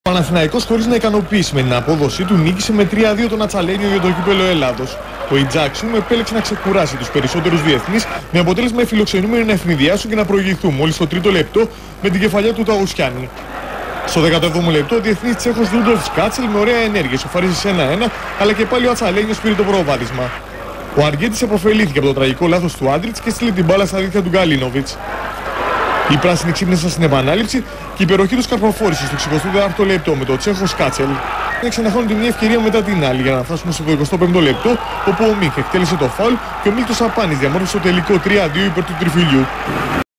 ΠΑΝΑΘΗΝΑΪΚΟΣ – ΑΤΣΑΛΕΝΙΟΣ 2004/05
ΗΜΕΡΟΜΗΝΙΑ :  10 ΣΕΠΤΕΜΒΡΙΟΥ 2004
ΔΙΟΡΓΑΝΩΣΗ : ΚΥΠΕΛΛΟ Α' ΓΥΡΟΣ Α' ΑΓΩΝΑΣ
ΓΗΠΕΔΟ :  ΑΠΟΣΤΟΛΟΣ ΝΙΚΟΛΑΪΔΗΣ - ΛΕΩΦΟΡΟΣ
ΑΠΟΤΕΛΕΣΜΑ : 3-2
ΣΚΟΡΕΡΣ : SKÁCEL 18' και 64' ΣΑΠΑΝΗΣ 85' – ΤΑΟΥΣΙΑΝΗΣ 3’ ΑΡΓΕΝΤΗΣ 25’
ΕΙΣΙΤΗΡΙΑ : 5.036